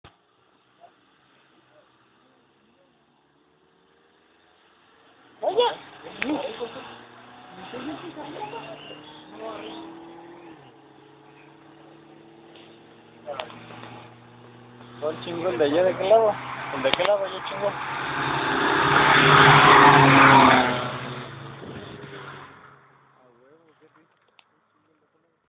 Golf y Caribe
NO IMPORTAN LOS RINES NI LUCES NI GRAN SONIDO DE ESTA CARIBE ANTE LA VELOCIDAD DE LA GOLF